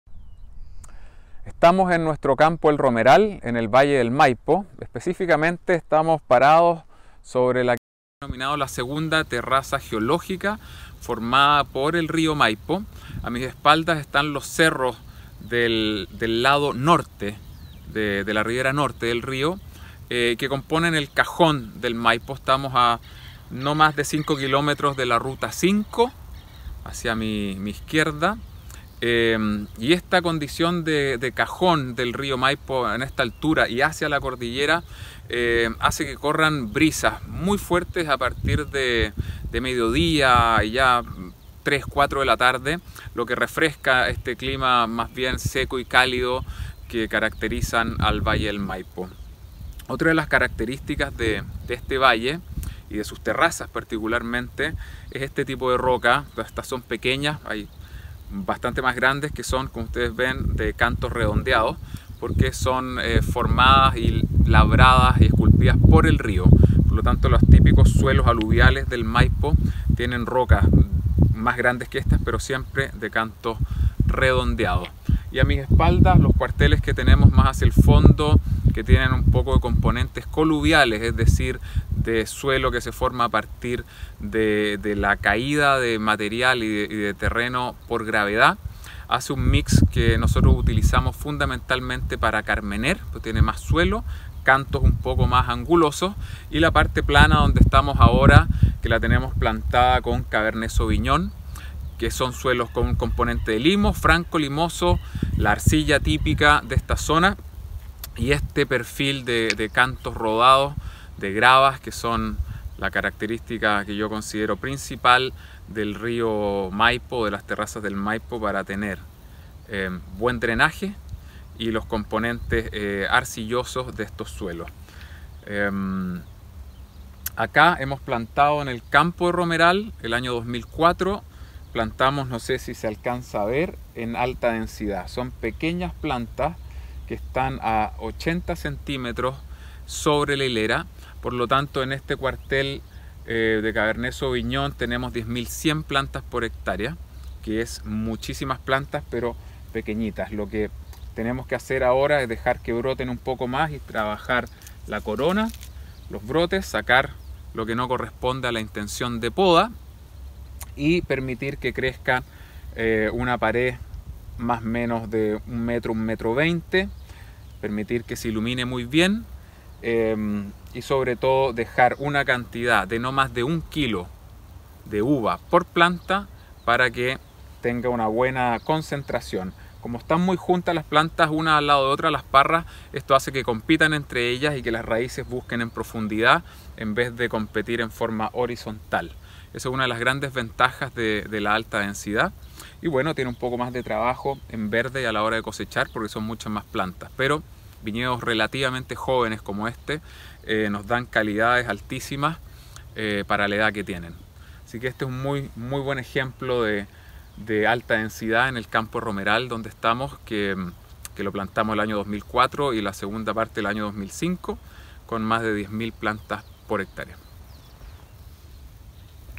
Campo El Romeral Valle del Maipo
Estamos en nuestro campo El Romeral, en el Valle del Maipo,
Específicamente estamos en parados sobre que es denominada la segunda terraza geológica,
Formada por el río Maipo,
A mis espaldas están los cerros del lado norte, de la rivera norte del rio,
Que componen el Cajón del Maipo,
Estamos a nomas de 5 kilómetros, de la ruta 5, hacia mi izquierda,
Esta condición de Cajón del rio Maipo en esta altura y hacia la cordillera
Hace que corran brisas muy fuertes a partir de mediodía, 3-4 de la tarde,
Lo que refresca, a este clima más bien seco y cálido,
Que caracterizan al valle del Maipo
Otra de las características de este valle y de sus terrazas particularmente,
Es este tipo de rocas, estas son pequeñas, hay bastante más grandes,
Que son de canto redondeados, porque son formadas y labradas por el rio
Por lo tanto, los típicos suelos aluviales del Maipo tienen rocas más grandes que estas
Pero siempre de canto redondeados,
A mi espalda, los cuarteles que tenemos más hacia el fondo
que tienen un poco de componentes coluviales, es decir de suelo que se forma a partir
de la caída del material y de terreno por gravedad,
hace un mix que nosotros utilizamos fundamentalmente para carmenere
porque tiene más suelo, cantos un poco más angulosos
y la parte plana en donde estamos ahora, que la tenemos plantada con cabernet Sauvignon
que son suelos con componentes limo franco limosos, la arcilla típica de esta zona
y este perfil de cantos rodados, de gravas que son la característica que yo considero principal
del río Maipo, de las terrazas del Maipo para tener buen drenaje,
y los componentes arcillosos de estos suelos
acá hemos plantado en el campo El Romeral el año 2004
no sé si se alcanza a ver, en alta densidad, son pequeñas plantas que van a 80 cm sobre la hilera
por lo tanto, en este cuartel de cabernet Sauvignon tenemos 10.100 plantas por hectárea
que es muchísimas plantas, pero pequeñitas,
lo que tenemos que hacer ahora es que broten un poco más y trabajar la corona,
los brotes, sacar lo que no corresponde a la intención de poda
y permitir que crezca una pared de más o menos 1 metro 20 cm
permitir que se ilumine muy bien,
y sobre todo dejar una cantidad de no mas de 1 kilo de uva por planta
para que tenga una buena concentración
cómo están muy juntas las plantas, una al lado de otra, las parras,
esto hace que compitan entre ellas y que las raíces busquen en profundidad
en vez de competir en forma horizontal,
esa es una de las grandes ventajas de la alta densidad
y bueno tiene un poco más de trabajo en verde y a la hora de cosechar
pero viñedos relativamente jóvenes como este
nos dan calidad altísima para la edad que tienen
así que este es un muy buen ejemplo de alta densidad en el campo romeral donde estamos
que lo plantamos el año 2004 y la segunda parte el año 2005 con mas de 10 mil plantas por hectárea.